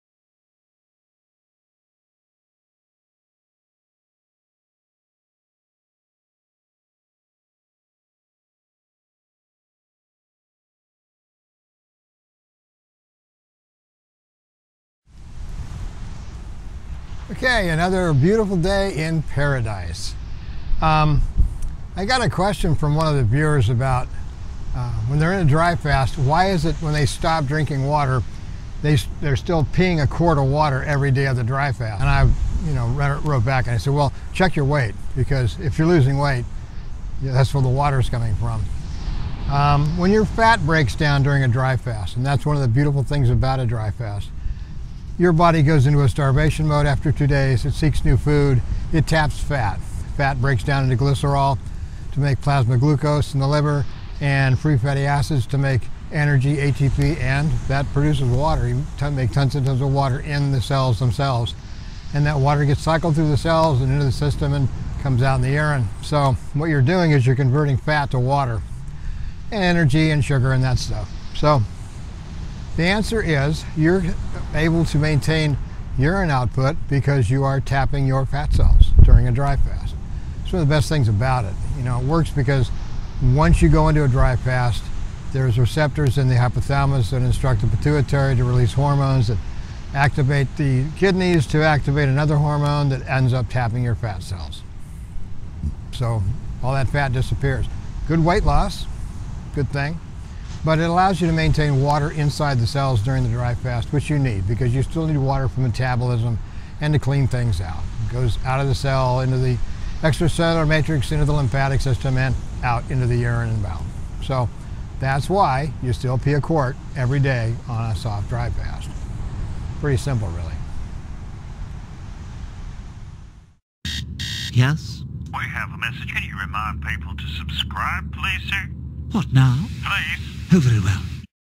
Urine Output During a Dry Fast
During a dry fast fat cells are tapped to keep plasma glucose available and to make energy with free fatty acids.  The free fatty acids make ATP and during that process make endogenous water inside every cell in the body. The result is no thirst but also normal urine output continues.  Fat deposits are reduced, and kidney function is normal.